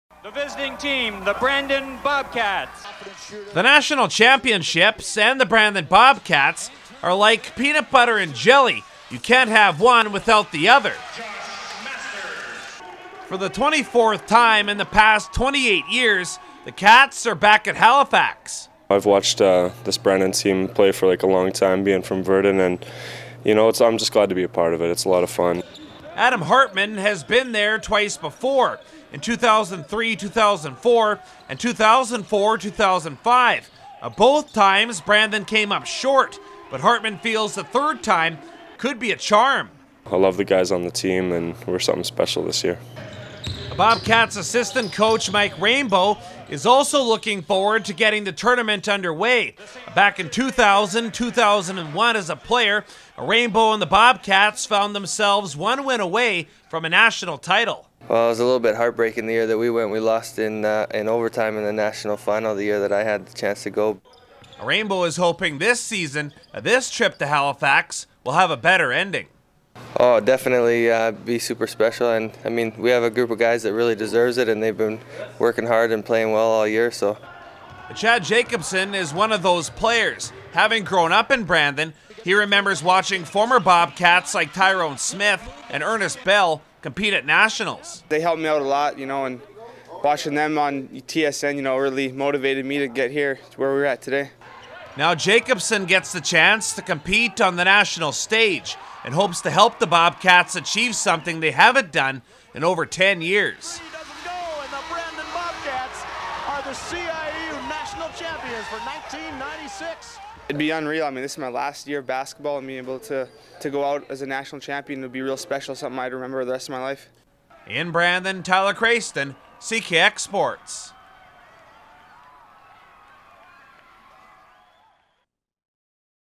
Bobcat Flashback: Men's basketball bound for 2007 nationals
Bobcat players Adam Hartman and Chad Jacobson along with BU assistant coach Mike Raimbault are interviewed in a CKX report as the men's basketball team departs for 2007 nationals.